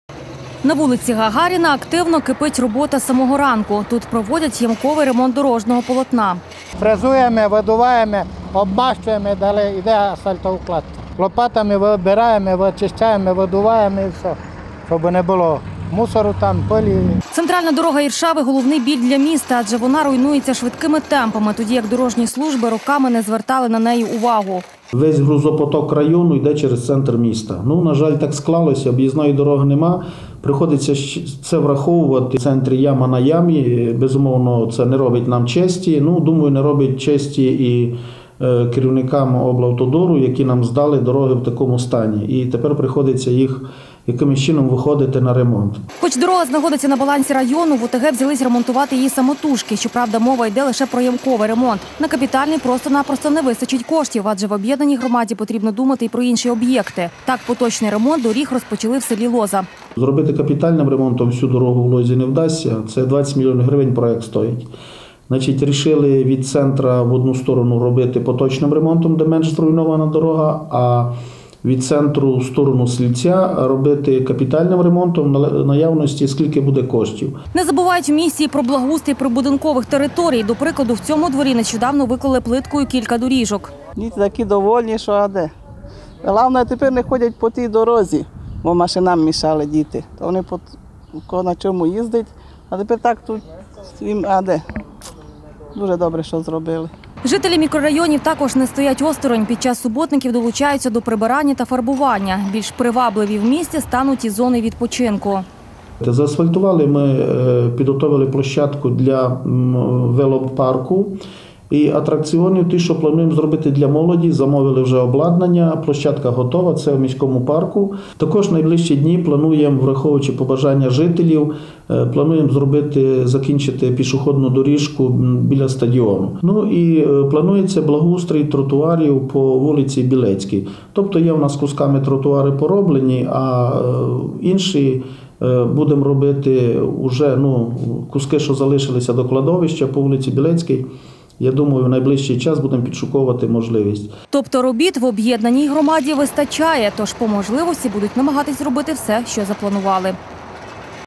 Ремонт та благоустрій:приємні зміни в ОТГ
На вулиці Гагаріна активно кипить робота з самого ранку. Тут проводять ямковий ремонт дорожнього полотна. Центральна дорога Іршави – головний біль для міста. Адже вона руйнується швидкими темпами, тоді як дорожні служби роками не звертали на неї увагу. Хоч дорога знаходиться на балансі району, в ОТГ взялись ремонтувати її самотужки. Щоправда мова іде лише про ямковий ремонт. На капітальний просто на просто не вистачить коштів. Адже, в об’єднаній громаді потрібно думати і про інші об’єкти. Так, поточний ремонт доріг розпочали в селі Лоза.Не забувають в місті і про благоустрій прибудинкових територій. До прикладу, в цьому дворі нещодавно виклали плиткою кілька доріжок.Жителі мікрорайонів також не стоять осторонь, під час суботників долучаються до прибирання та фарбування. Привабливішими в місті стануть і зони відпочинку. Тобто робіт в об’єднаній громаді вистачає.Тож, по можливості будуть намагатись зробити все, що запланували.